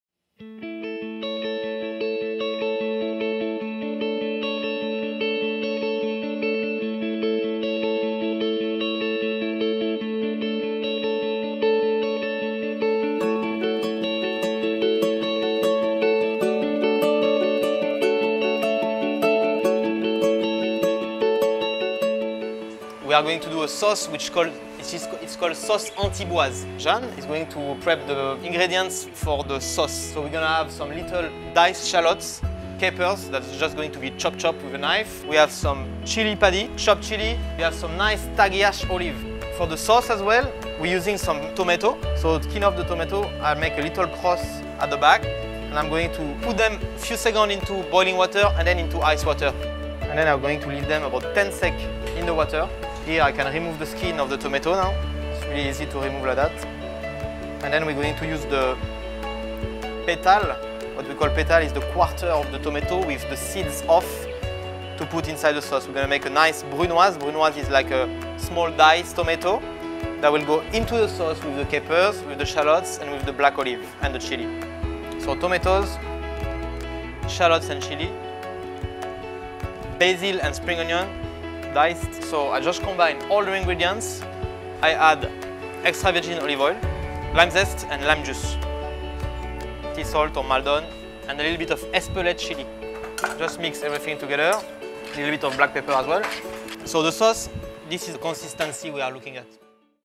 Julien Royer's Confit & Smoked Barramundi Recipe (Antiboise Sauce Preparation)
INGREDIENTS:
- 3 pcs Rabelais tomatoes or any good quality ripe tomatoes
- 1 head of fresh spring onions
- 2 Table spoon of capers
- 1/2 Chili padi
- 5 large leaves of Basil
- 1 pcs of fresh lime
- 2 table spoon of ‘Taggiasca’ olives
- 100 gr extra virgin olive oil
- 1 pcs of banana shallots
- 1 pcs Garlic clove
- Salt/pepper/Espelette

 METHOD:
- Peel the tomatoes, remove seeds and cut a nice, neat and regular
  ‘brunoise’
- Chop the capers and black olive roughly.
- Slice thinly spring onions and basil,
- Zest and Juice the lime.
- Chop the chili padi very thinly and remove seeds.
- Dice  shallots very thinly.
- Combine all ingredients in a bowl, season with fine salt, black
   pepper and Espelette chili.